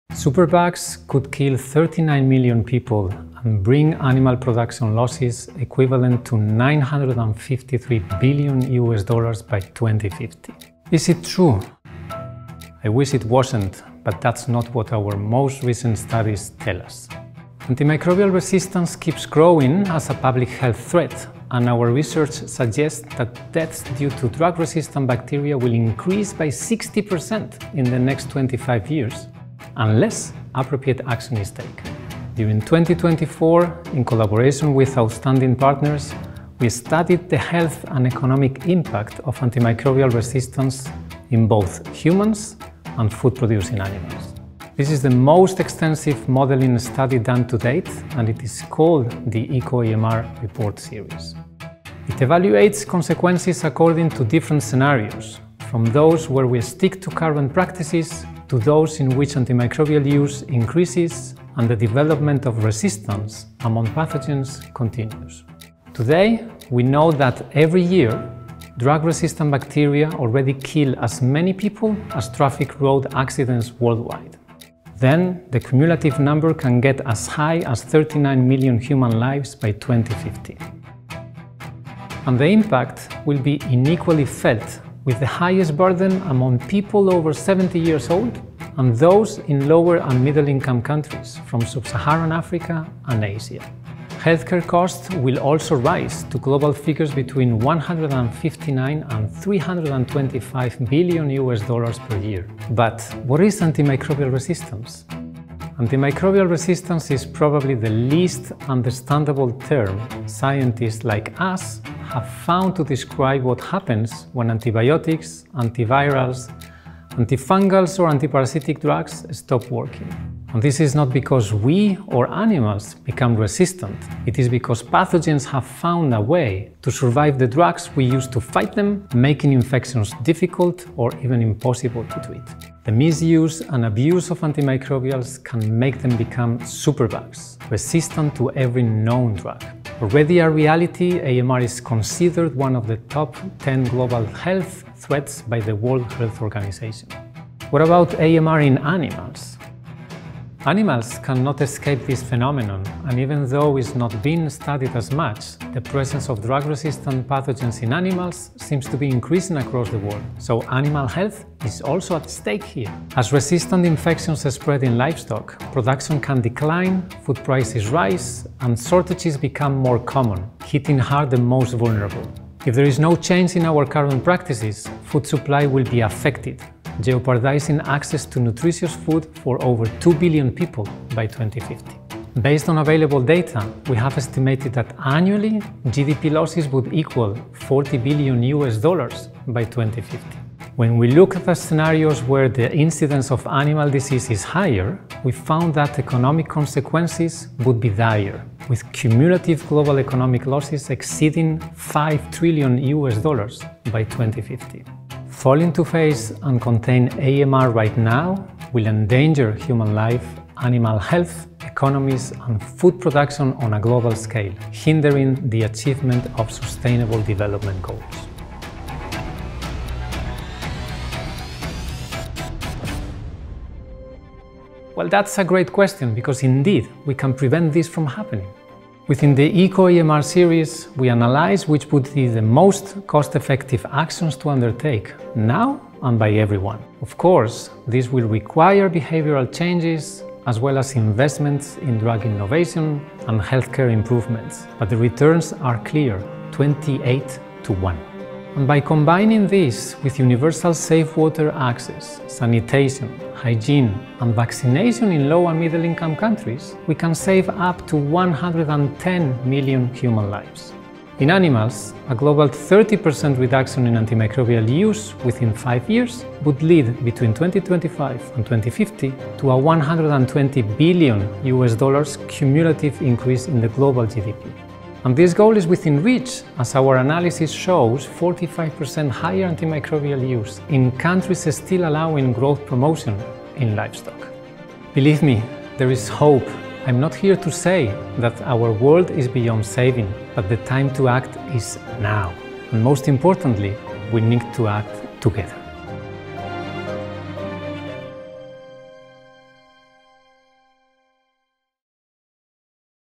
The deathly cost of drug-resistant infections
Superbugs are not a distant threat. By 2050, antimicrobial-resistant “superbugs” could threaten food security for more than two billion people and drive global health care costs up by US$159 billion every year.

Across the world, microorganisms (bacteria, viruses, fungi and parasites) are becoming resistant to the very drugs used to fight them, in animals and humans. This is antimicrobial resistance or AMR. When these pathogens no longer respond to medicines, they turn into “superbugs”, making infections harder and sometimes impossible to treat. Antimicrobial resistance is greatly accelerated by the improper use and misuse of antimicrobials.